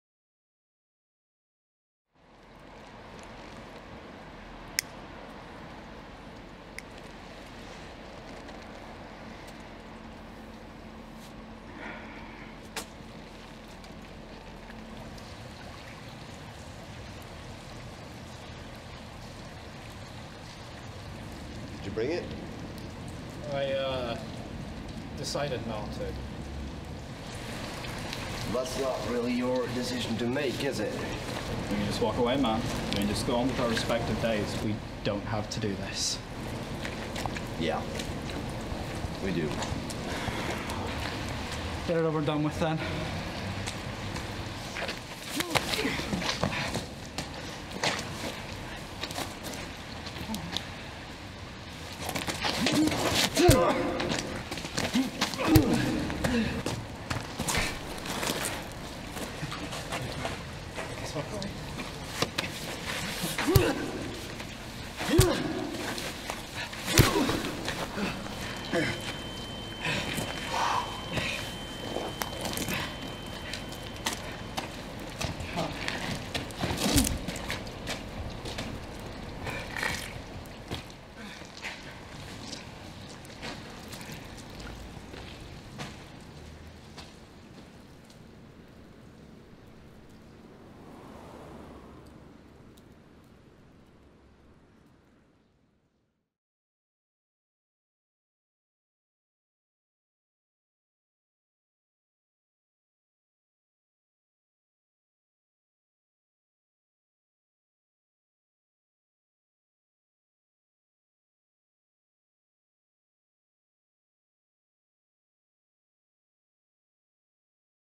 Fight Film | Scene Experiment | Editor & Gaffer
This film was created after given the prompt/theme as "Sketches". So, our group created a fight sketch as it is something we had neve done, but have all wanted to do.